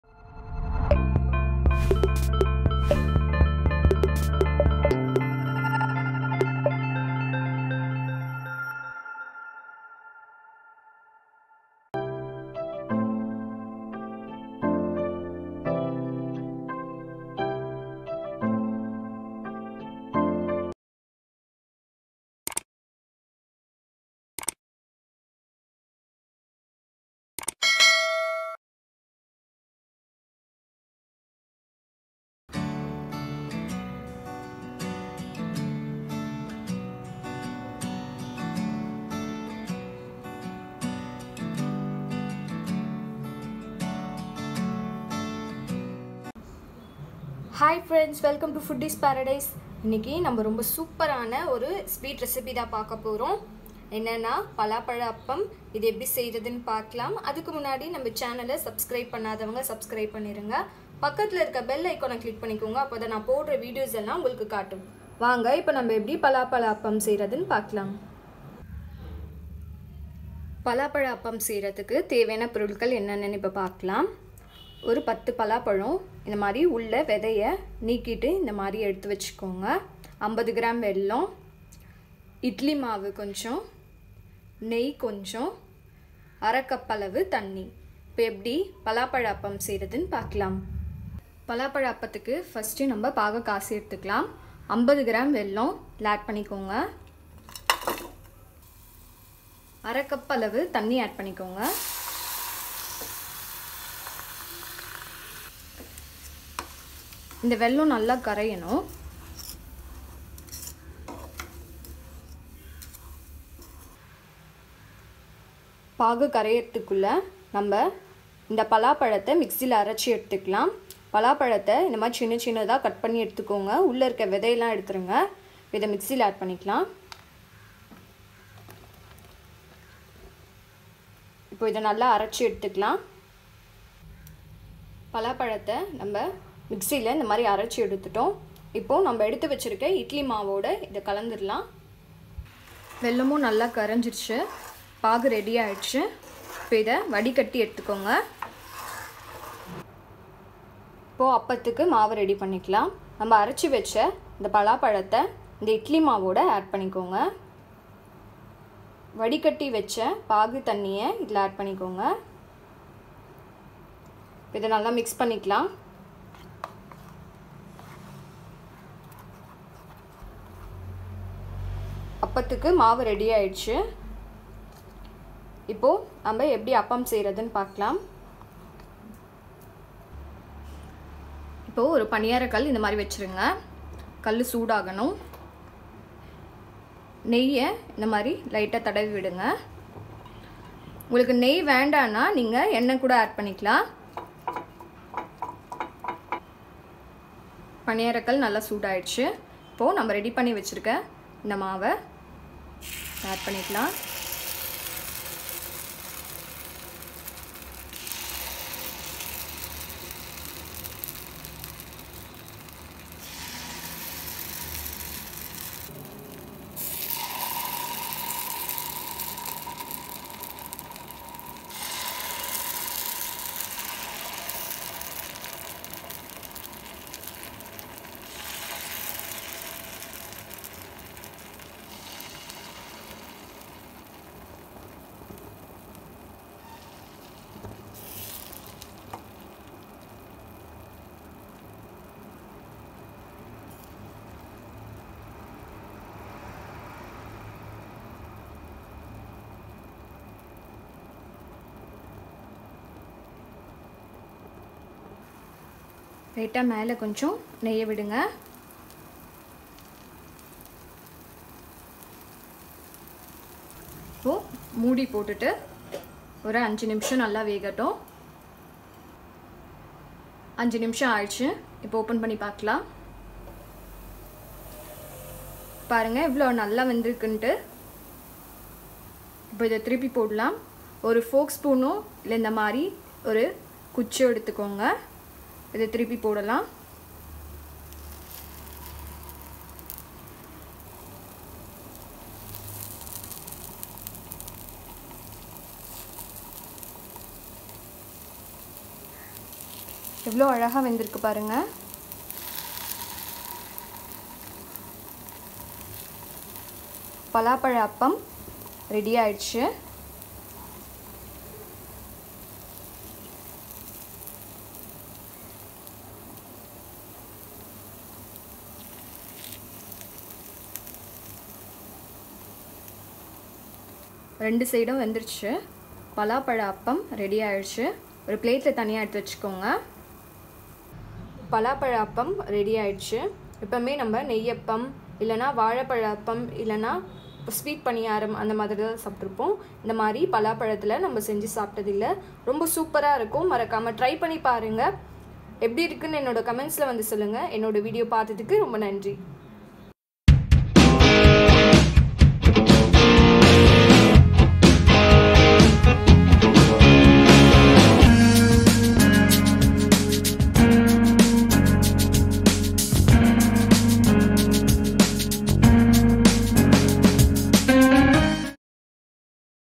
Palapazha Paniyaram Recipe in Tamil|Palapazha Appam Recipe in Tamil|Jackfruit Paniyaram Recipe
Chakka Nei Appam is a sweet, flavorful and tasty Appam made using rice flour, jaggery and ripe Jackfruit. It’s a conventional Nei Appam, but with the addition of Jackfruit but the addition of Jackfruit brings very good fragrance and flavor to the appam. It’s very common in Kerala as the Jackfruits are abundant there.

These Chakka Nei Appam are traditional sweets which are easy to make. All you need is a Appam Pan (Paniyaram Pan), the basic ingredients like Rice flour, jaggery and few jackfruit to make this flavorful appam. This appam can also be made using Chakka Varatti . Using Varatti this appam is more tasty because of the concentrated fruit pulp. In this version i have given an instant version of making appam using fresh Jack fruit

INGREDIENTS

Jackfruit-10 to15Nos
Jaggery-50gm
Idly/Dosa batter
Water-1/2Cup
Ghee/Oil-ur choice